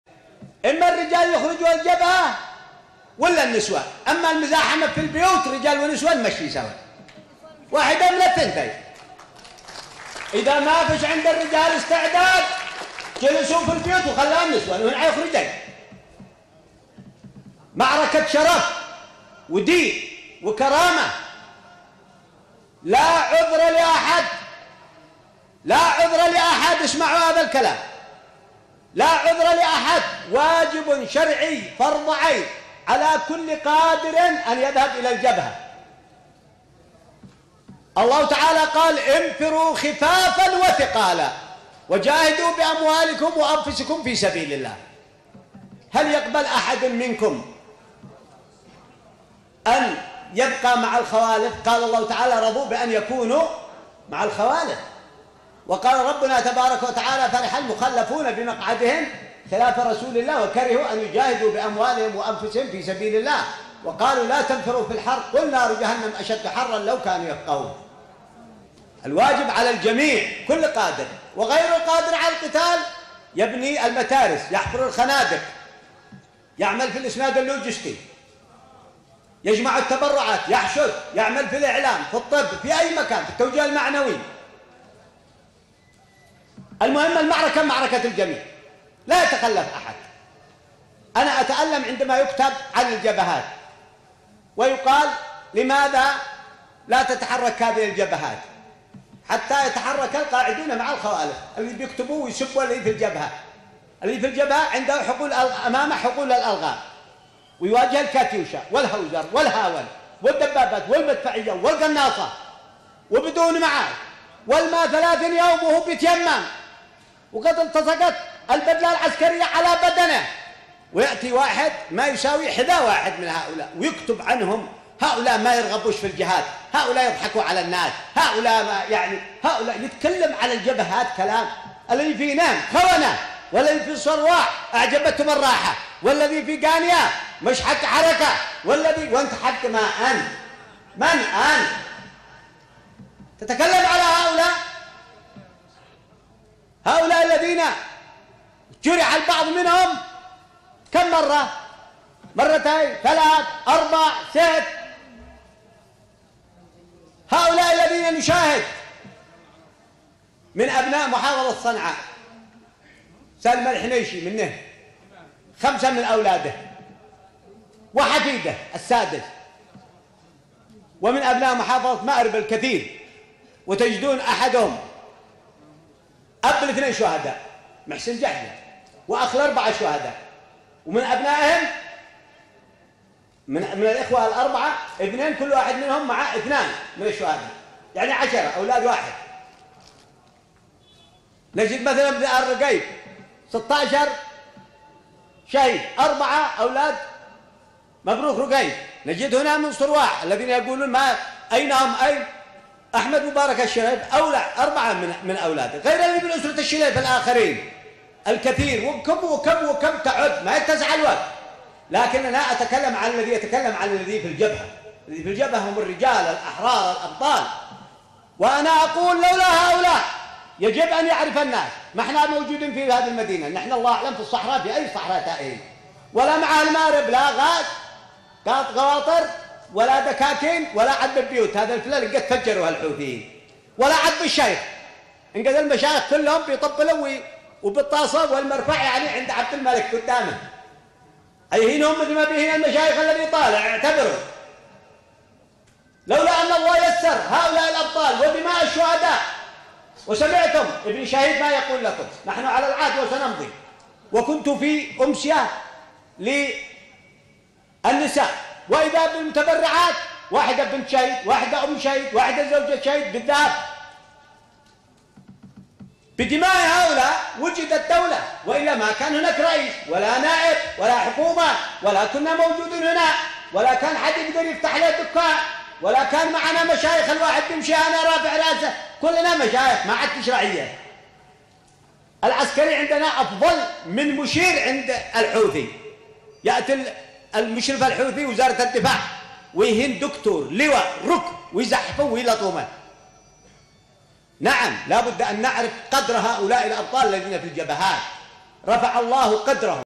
الشيخ عبدالله صعتر مواجهة الحوثيين واجب على الجميع مارب اليمن
اكد الشيخ عبدالله صعتر في كلمته بالأمسية الرمضانية لأبناء محافظة صنعاء ان مواجهة مليشيات الحوثيين واجب على الجميع و لا عذر لأحد .
27 مايو 2019

مارب
اليمن

Abdrhman Almasri

Yemen

Marib

لمشاهدة مقاطع فيديو بقناتي باليوتيوب اضغط على الرابط التالي